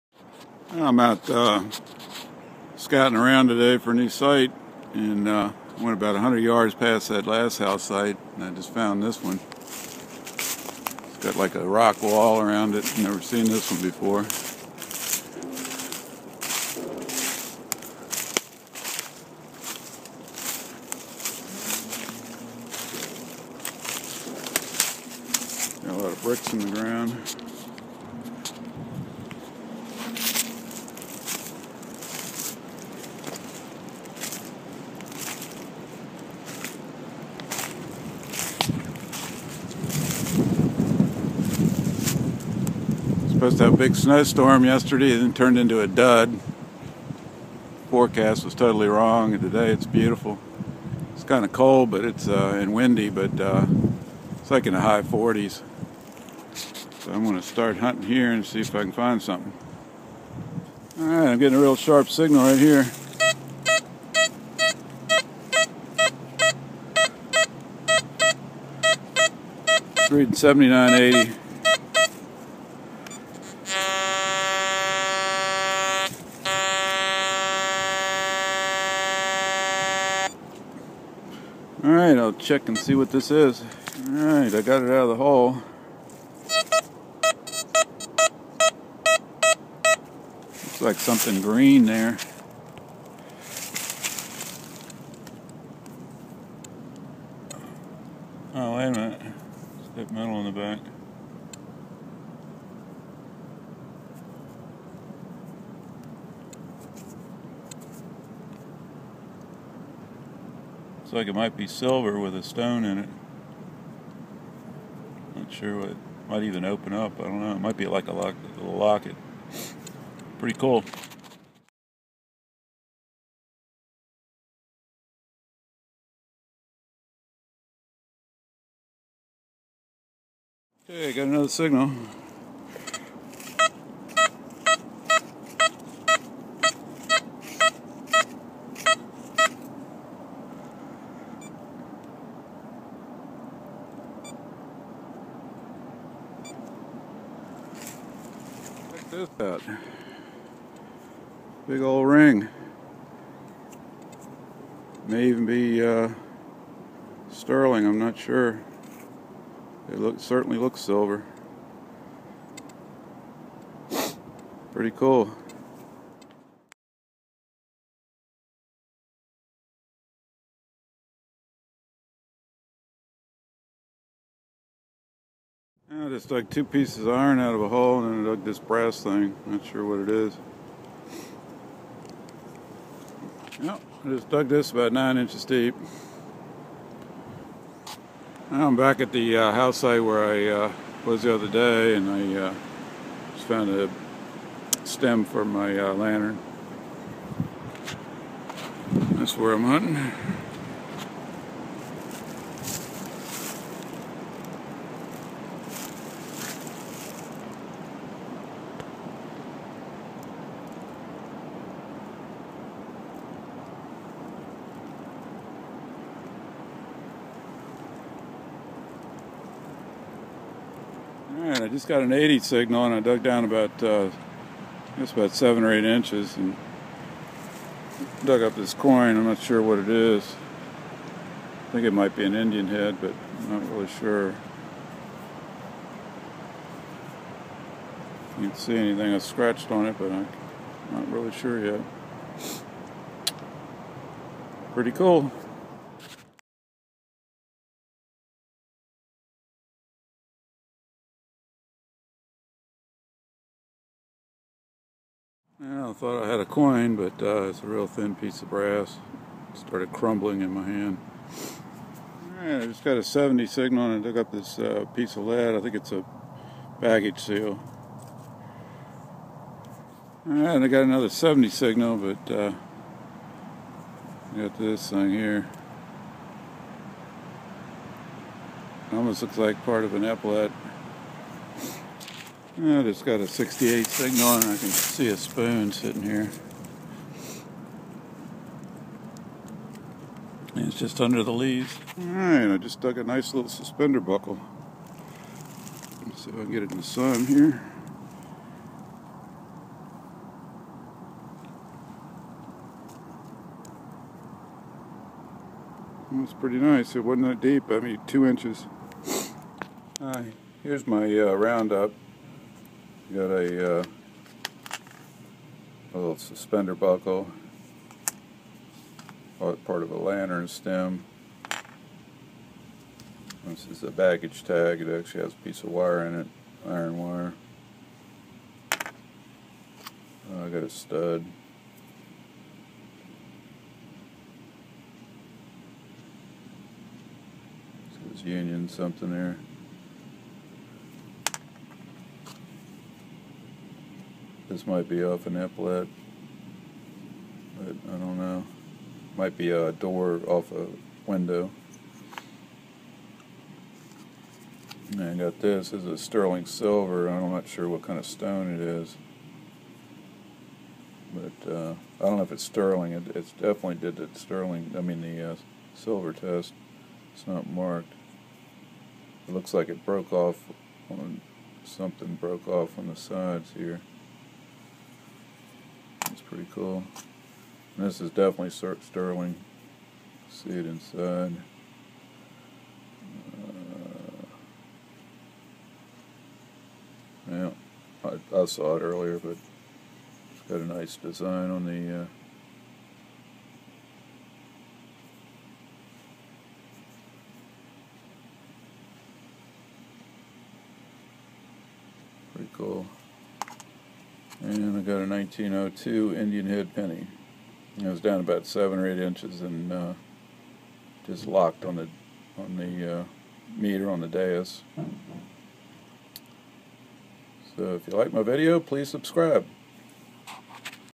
XP Deus Finds IHC & Silver Jewelry
Found a new house site 100 yards past the site I detected two days ago. Found keepers at both sites. Using the XP Deus today I like the machine a lot.